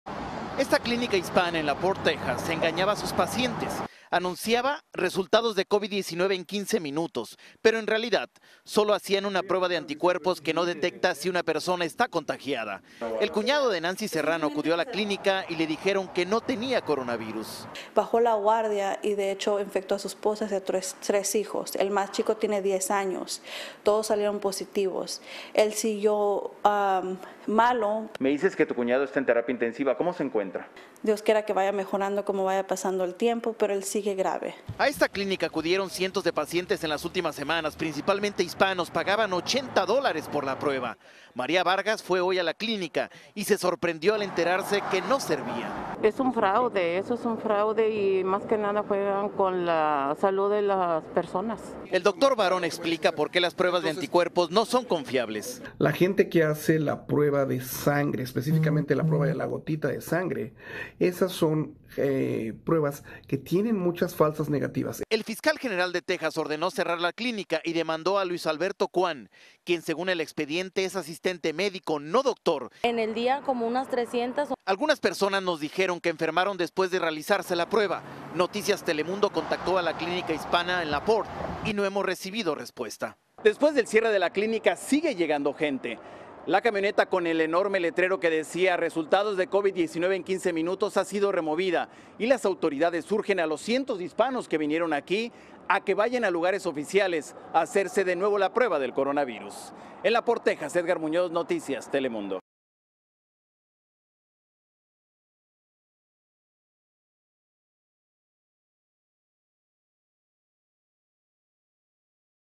Cierran clínica hispana en Texas por hacer pruebas de COVID-19 fraudulentas | Noticias Telemundo
Video oficial de Noticias Telemundo. El centro de salud ofrecía diagnósticos rápidos de coronavirus usando pruebas presuntamente falsas, ahora muchos de los pacientes que atendieron han resultado infectados con el virus.